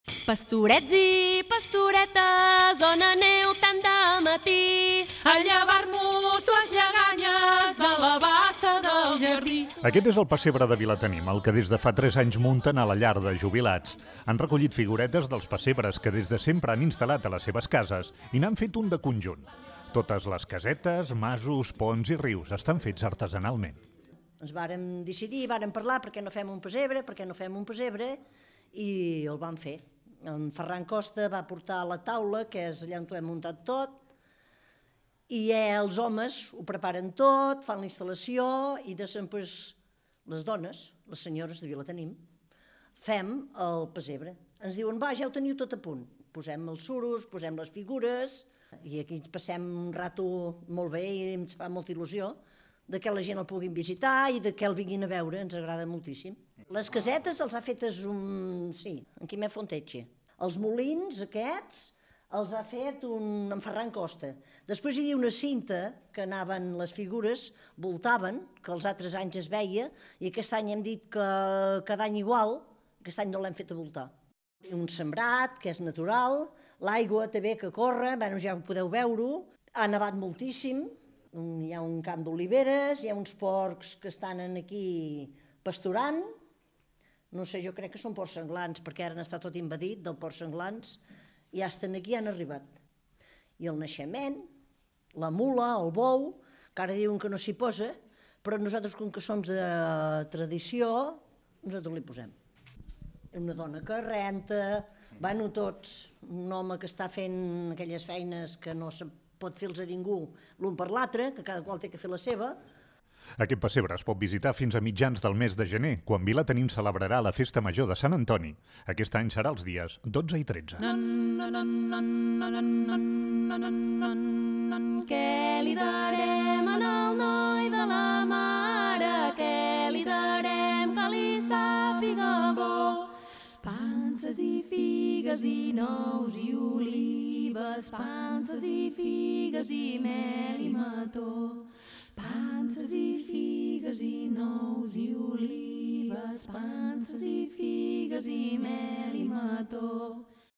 Pessebre de Vilatenim
Ja fa tres anys que la Llar de Jubilats de Vilatenim instal·la un gran pessebre. Els homes fan les castes de fusta, els ponts i les masies i les dones hi distribueixen les figuretes. Es pot visitar fins a mitjans de gener.